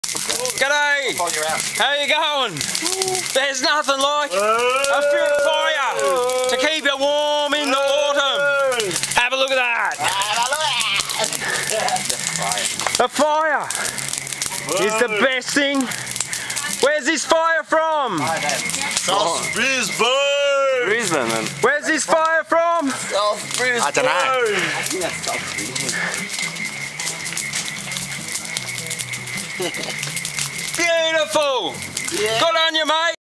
There's nothing like Australia and a fire in the backyard to keep you warm
There's nothing like Australia and a big fire in the back yard to keep you warm in the Autumn, with your crazy mates.